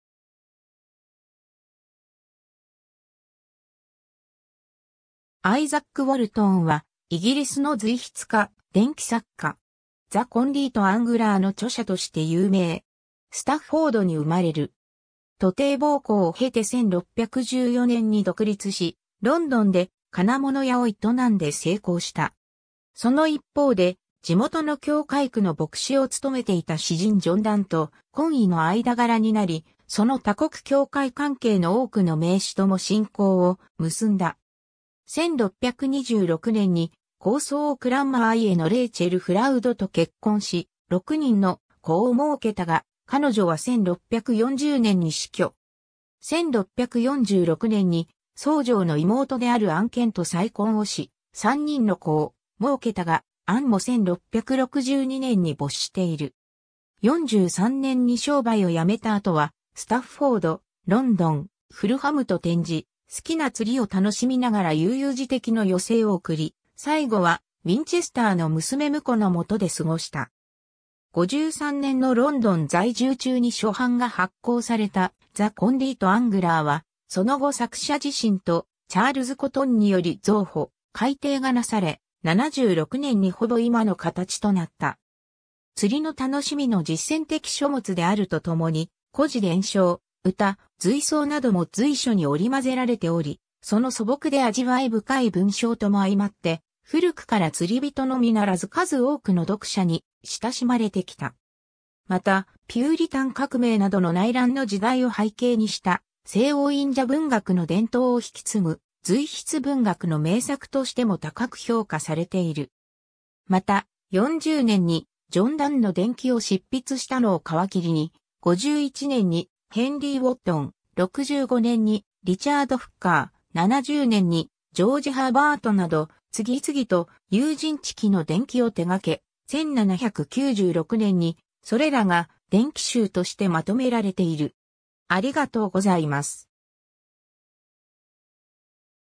アイザック・ウォルトン
アイザック・ウォルトン（Izaak Walton, 1593年8月9日 - 1683年12月15日）は、イギリスの随筆家、伝記作家。
"The Compleat Angler"（『釣魚大全』）の著者として有名。
 スタッフォードに生まれる。
徒弟奉公を経て1614年に独立し、ロンドンで金物屋（一説には呉服屋）を営んで成功した。
その一方で、地元の教会区の牧師を務めていた詩人ジョン・ダンと懇意の間柄になり、その他国教会関係の多くの名士とも親交を結んだ。
1626年に高僧クランマー家のレイチェル・フラウドと結婚し、6人の子をもうけたが、彼女は1640年に死去。
1646年に僧正の妹であるアン・ケンと再婚をし、3人の子をもうけたが、アンも1662年に没している。
43年に商売を辞めた後は、スタッフォード、ロンドン、フルハムと転じ、好きな釣りを楽しみながら悠々自適の余生を送り、最後はウィンチェスターの娘婿のもとで過した。
 53年のロンドン在住中に初版が発行された"The Compleat Angler"（『釣魚大全』）は、その後作者自身とチャールズ・コトンにより増補・改訂が為され、76年にほぼ今の形となった。
釣りの楽しみの実践的書物であるとともに、故事伝承・歌・随想なども随所に織りまぜられており、その素朴で味わい深い文章ともあいまって、古くから釣り人のみならず数多くの読者に親しまれてきた。
また、ピューリタン革命などの内乱の時代を背景にした、西欧隠者文学の伝統を引き継ぐ随筆文学の名作としても高く評価されている。
 また、40年にジョン・ダンの伝記を執筆したのを皮切りに、51年にヘンリー・ウォットン、65年にリチャード・フッカー、70年にジョージ・ハーバートなど、次々と友人知己の伝記を手がけ、1796年にそれらが『伝記集』としてまとめられている。